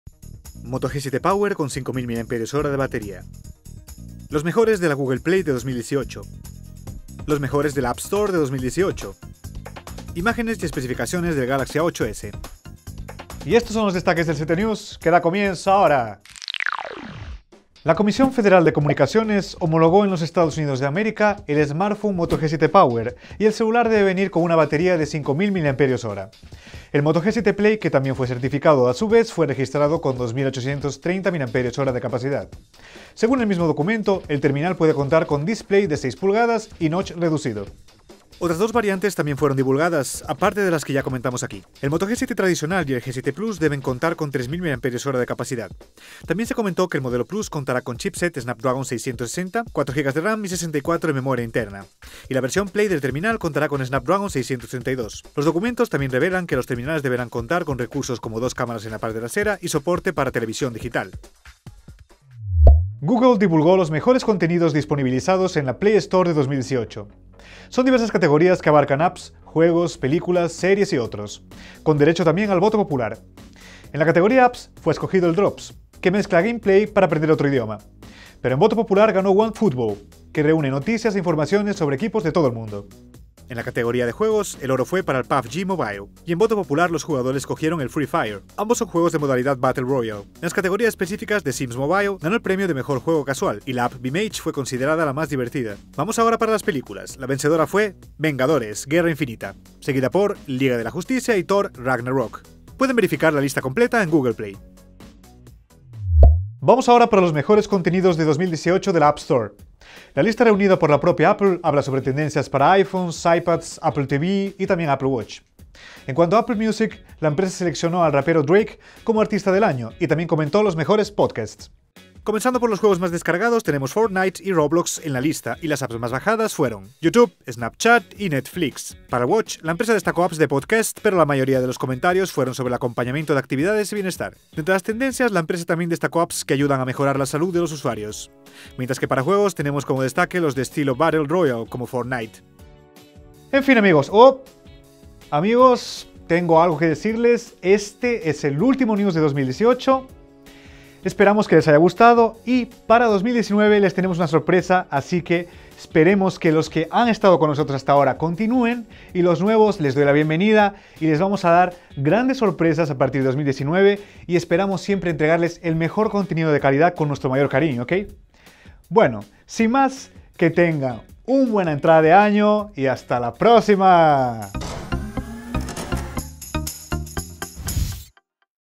Moto G7 Power con 5000 mAh; Los mejores de la App Store 2018 y + [Ct News]
DESTAQUES:
1. Los mejores de la Google Play 2018
2. Los mejores de la App Store 2018
3. Moto G7 Power con 5000 mAh de bateria

Siga Canaltech!